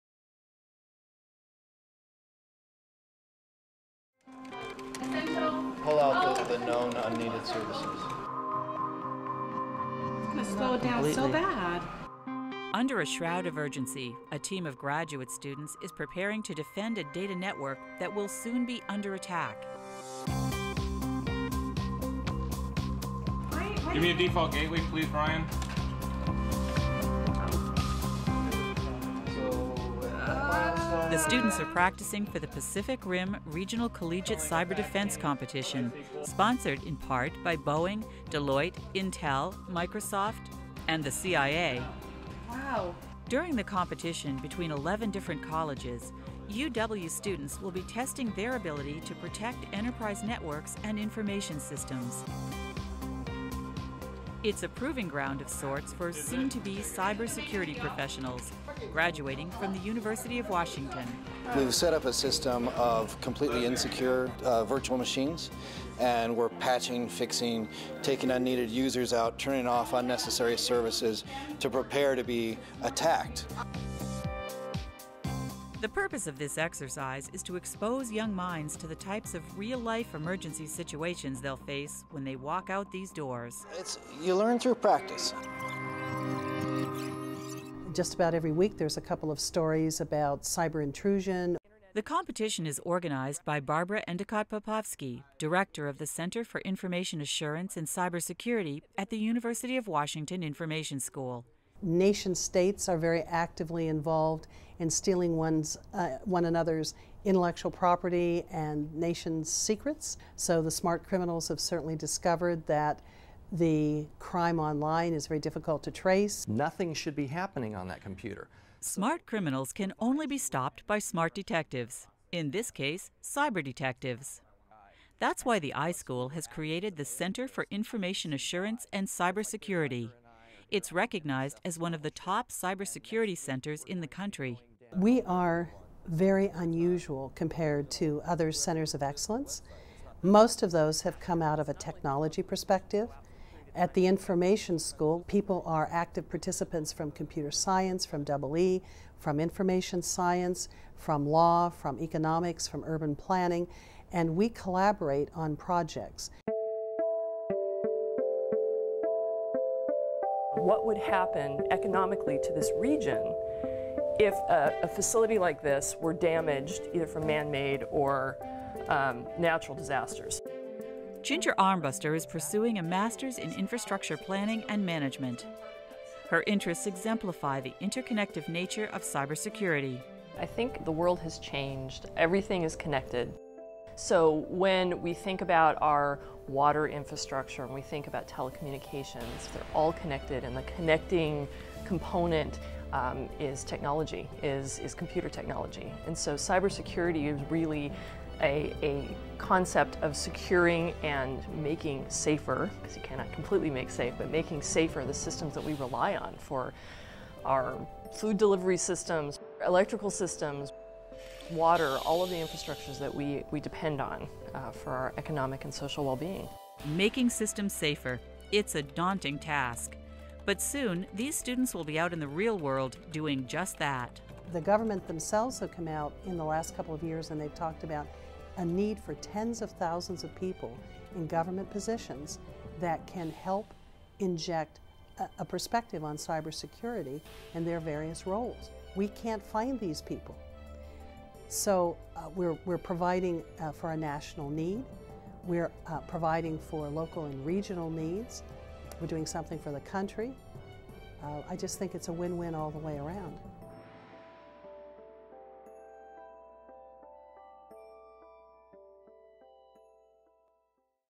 Cybersecurity & Information Assurance with Barbara Endicott-Popovsky
The Pacific Rim Regional Collegiate Cyber Defense Competition provides real world cybersecurity and information assurance experience that can't be replicated in a classroom. Teams of students compete against their peers and professionals in the cybersecurity with the winner of the competition advancing to the national championship.

This segment is part of a 60-minute UWTV-produced documentary film about the UW Information School and its work, which aims to draw attention to the school's faculty and the social impact of their research.